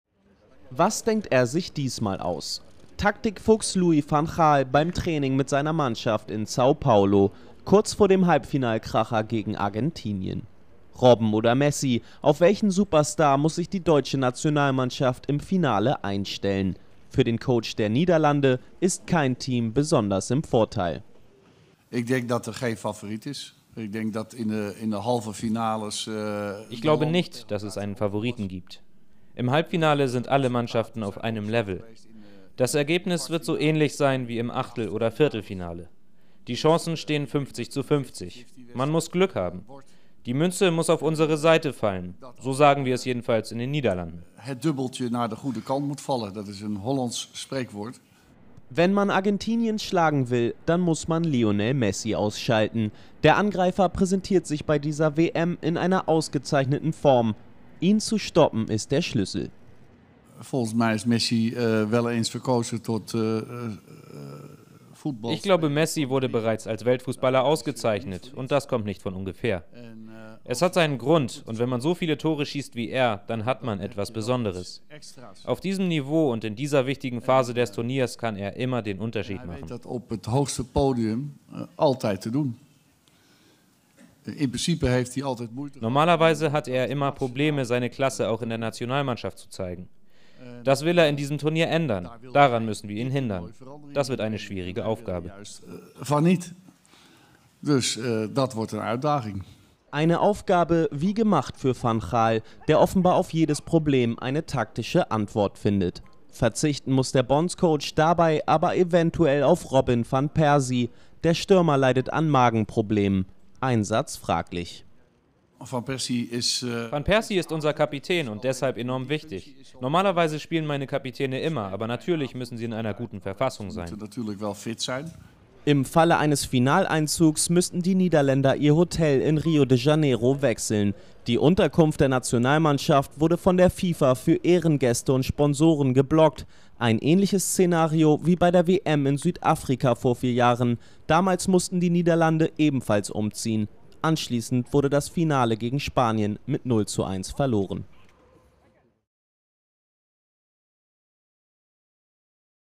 Mister Unberechenbar - Van Gaals Planspiele gegen Messi - kicker.tv
Alles hört auf sein Kommando: Bondscoach Louis van Gaal hat das alleinige Sagen bei der Elftal, Widerspruch lässt der 62-Jährige kaum zu. Spätestens nach dem 4:3 i.E. im Viertelfinale gegen Costa Rica fliegen dem "General" aber auch die Herzen in Holland zu. Welche taktischen Tricks lässt sich van Gaal für das WM-Halbfinale gegen Argentinien einfallen? Immerhin gilt es in einer Neuauflage des Endspiels von 1978, keinen Geringeren als Lionel Messi zu stoppen.